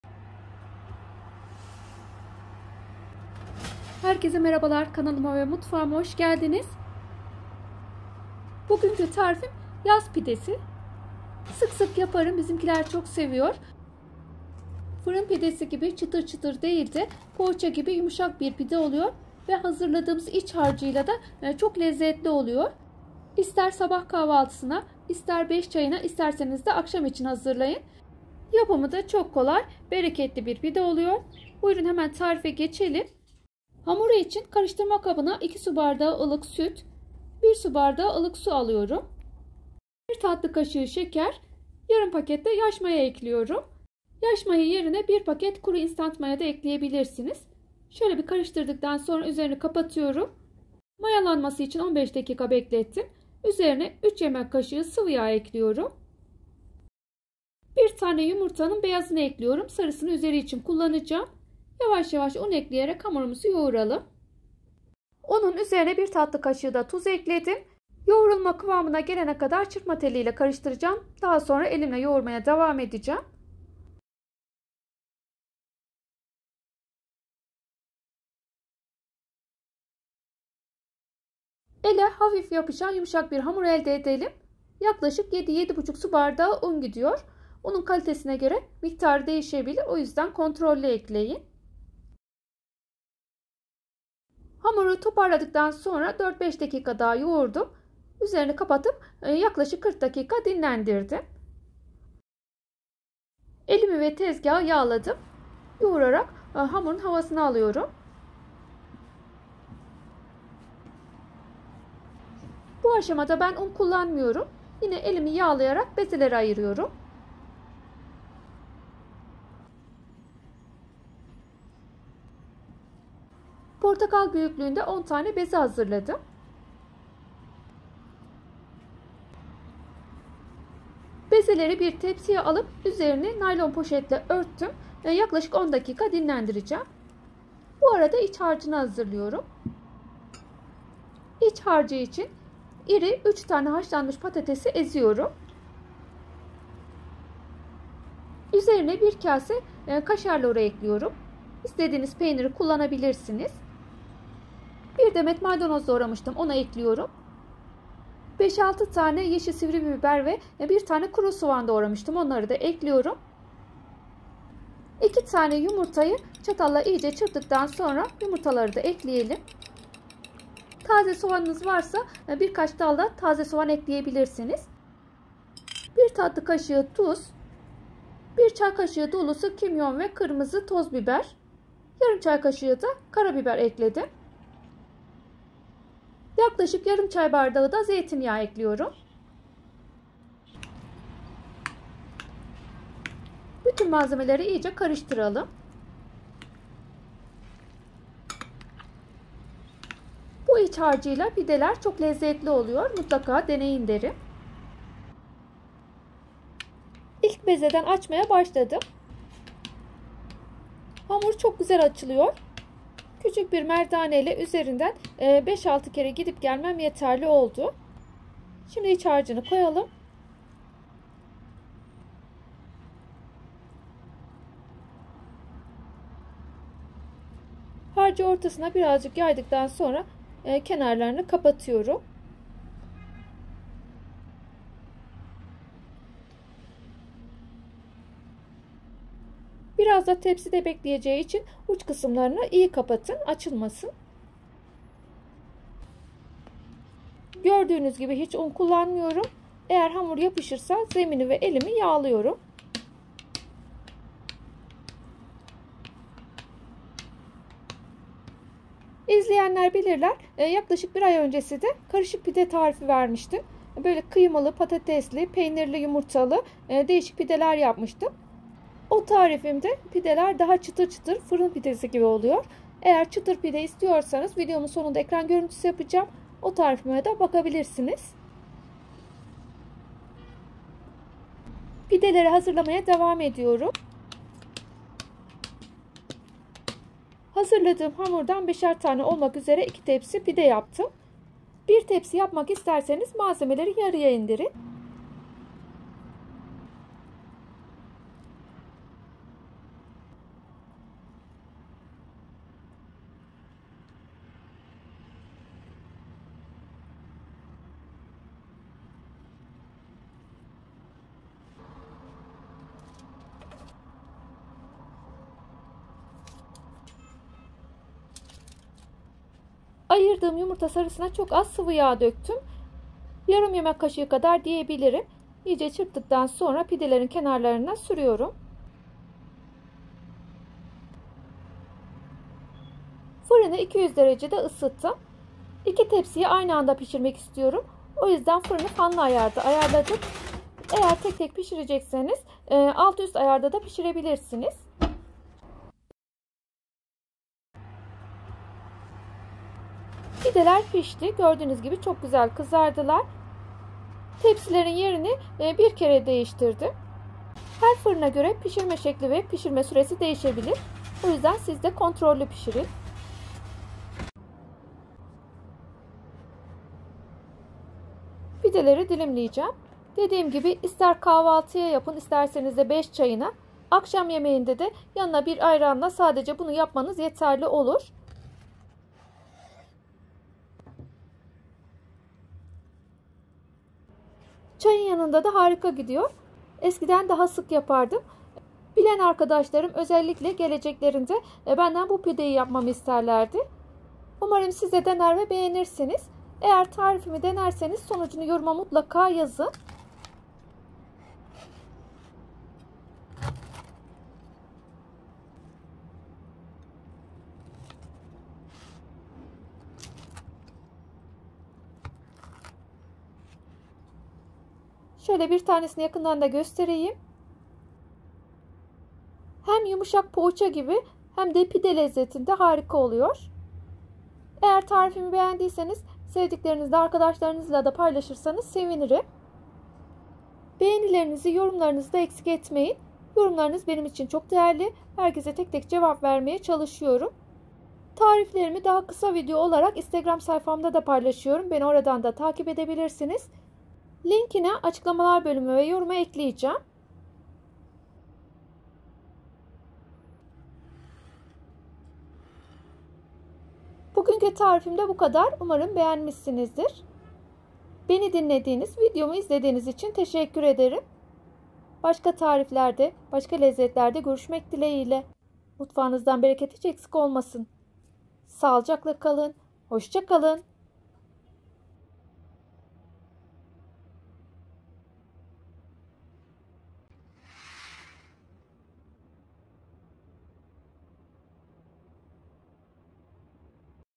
Yıllardır yaparım Arkadaşlarım 5 çayına benden KIR PİDESİ ister Lezzeti iç harcında saklı 😋
Malzemeler-

Hamuru için -
2 su bardağı ılık süt
1 su bardağı ılık su
1 tatlı kaşığı şeker
1 adet yumurta ( sarısı Üzerine)
3 yemek kaşığı sıvıyağ
Yarım paket yaş maya
1 tatlı kaşığı tuz
7-7,5 su un

3-4 adet haşlanmış patates
1 kase kaşar loru
1 adet kuru soğan
1 demet maydanoz
5-6 adet yeşil biber
2 adet yumurta
tuz, kimyon, karabiber

Beni instagramdanda takip edebilirsiniz Tariflerimi oradanda paylaşıyorum